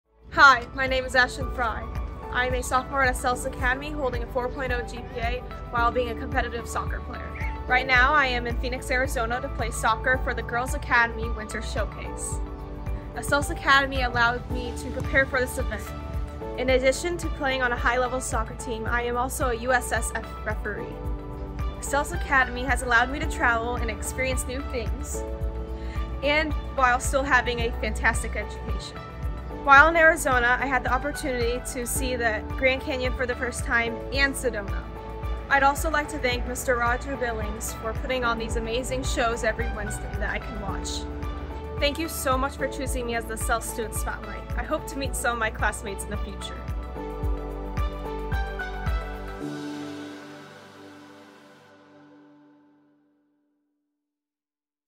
Meet Ashtyn - Acellus Academy's Soccer Star & Scholar
Meet Ashtyn, a talented sophomore holding a remarkable 4.0 GPA while excelling as a competitive soccer player. Join us as she takes us on an exciting journey to the Girls Academy Winter Showcase in Phoenix, Arizona, where she showcases her skills on the field and shares how Acellus Academy has played a crucial role in her success.

Ashtyn not only demonstrates her prowess on the soccer pitch but also reveals her passion for the sport as a USSF referee. Discover how Acellus Academy has empowered her to balance a high-level sports career with a fantastic education, enabling her to explore new places like the breathtaking Grand Canyon and captivating Sedona during her time in Arizona.

Looking to enroll your student in Acellus Academy?